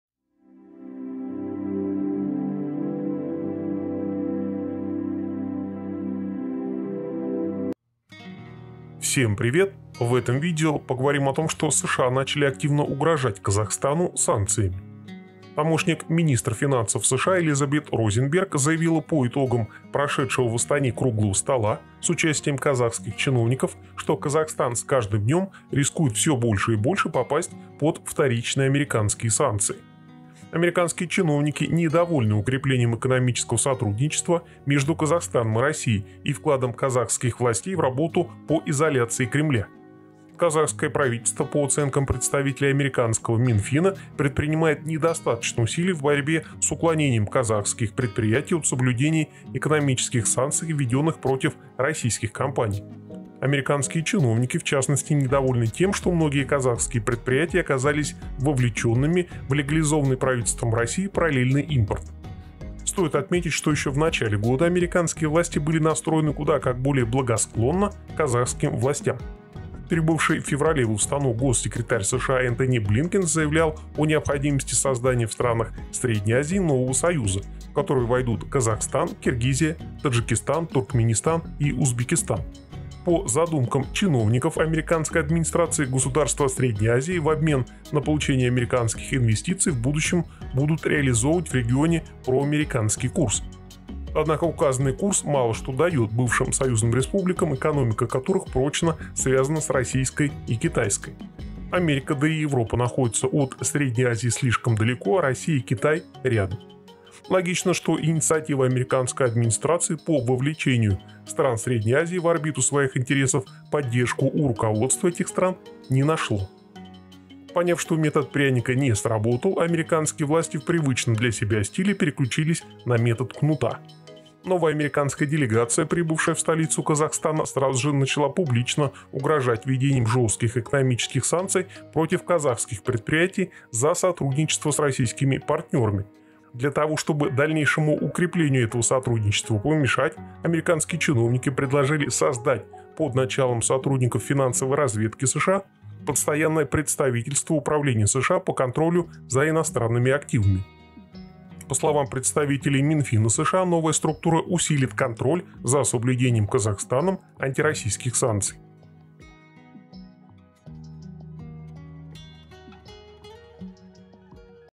США начали угрожать Казахстану санкциями
США начали угрожать Казахстану санкциями. США пригрозили руководству Казахстана введением вторичных санкций за сотрудничество с Россией. Власти США хотят, чтобы страны Средней Азии проводили проамериканский курс в регионе. США недовольны, что казахские предприятия участвуют в поставке санкционных товаров в Россию